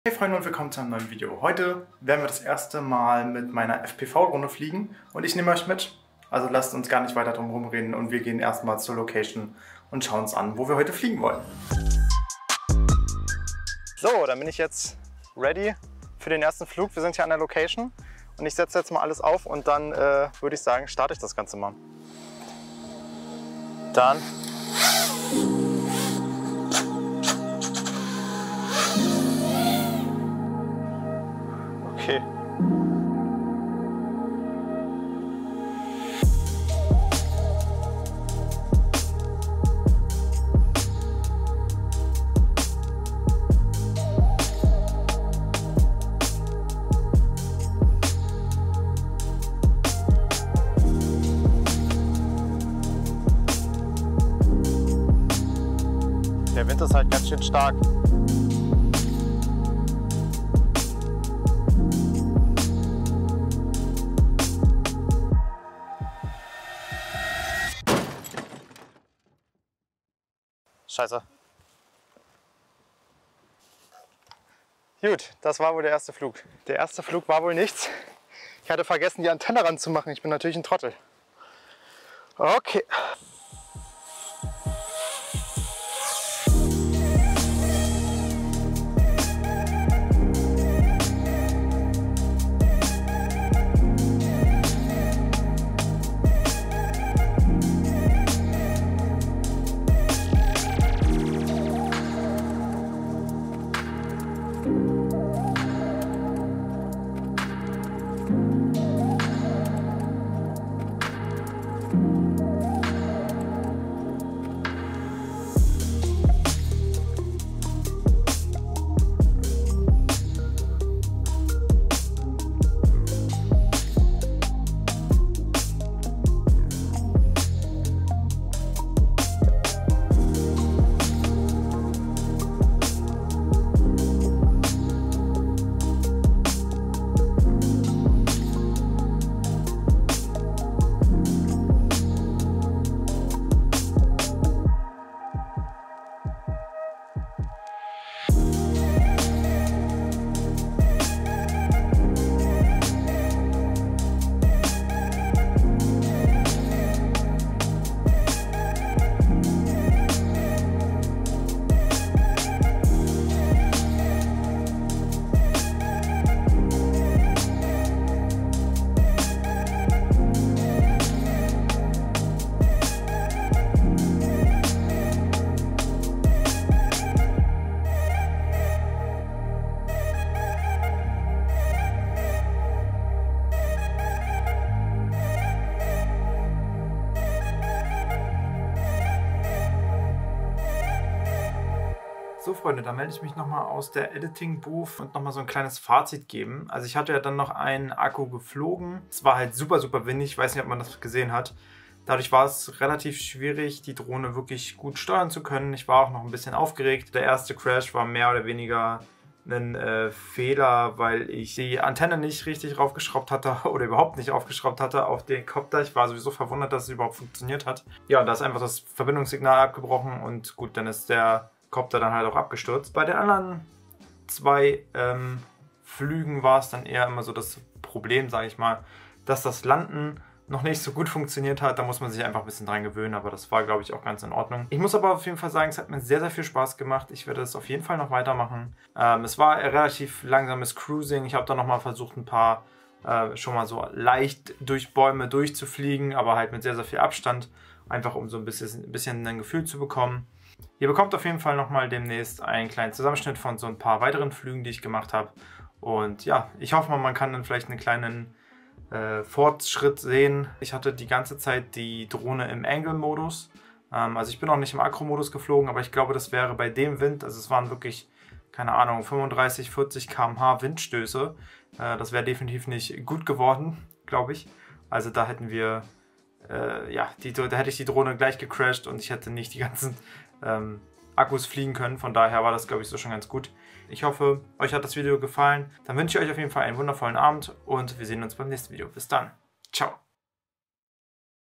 ERSTER FPV FLUG geht SCHIEF?! - FPV Flug mit Nazgul Evoque F5
Endlich ist es soweit und ich nehme euch mit zu meinem ersten FPV Flug. Beim nächsten Video gibt es auch gute Videoqualität von der GoPro, versprochen. Seht es mir bitte nach, dass die ersten Flugversuche vielleicht noch nicht so smooth aussehen. Es macht mir aber auf jeden Fall sehr viel Spaß und ich werde weiter an diesem Hobby dran bleiben und euch auf meinem Web mitnehmen.

Ich würde mich sehr über konstruktives Feedback freuen. Bitte bleibt respektvoll im Kommentarbereich.